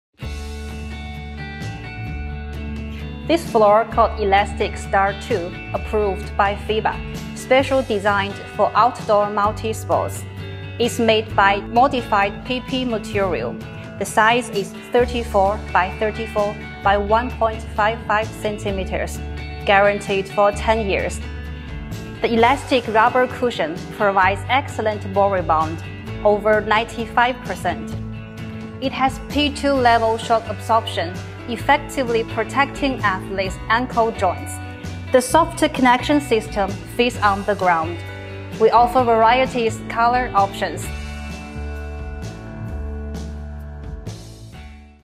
Elastic Star Ⅱ outdoor court tiles
Excellent all-weather play.
Competitively priced high performance athletic surface.
With elastic shock pads offering a better shock absorption performance for the players.
100% Food grade raw material.
Low maintenance.
Vertical and lateral shock absorption for player safety.
UV stable treatment keeps the court color fresh after longer time used
Excellent non-slip grip, prevent the displacement